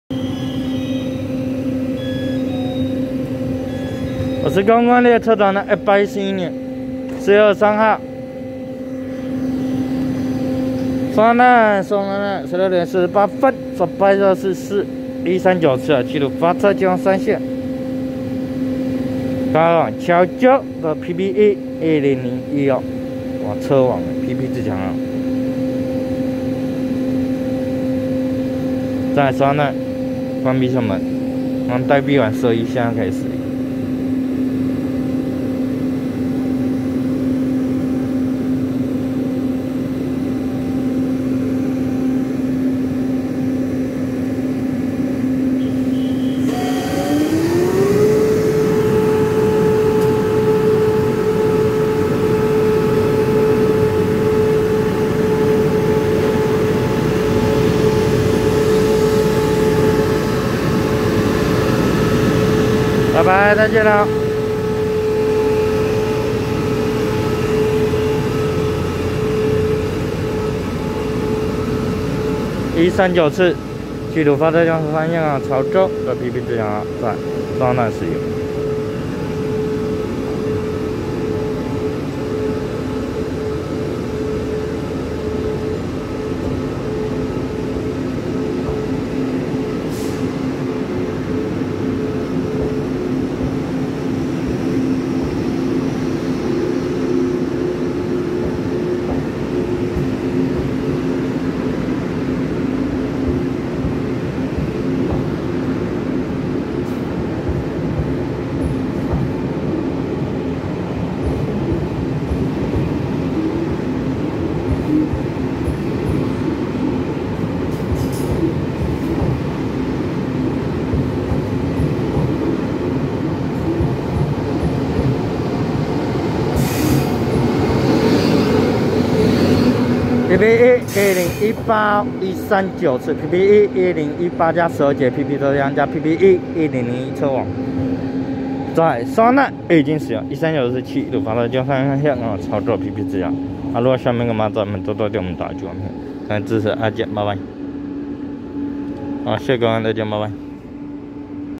139次七堵發車經由山線開往潮州的PPE1018單電豬PP電頭+十二節PP車廂+PPE1001單電豬PP電頭車王PP自強號
111/4/23松山站16:48分拍攝139次七堵發車經由山線開往潮州的PPE1018單電豬PP電頭+十二節PP車廂+PPE1001單電豬PP電頭車王PP自強號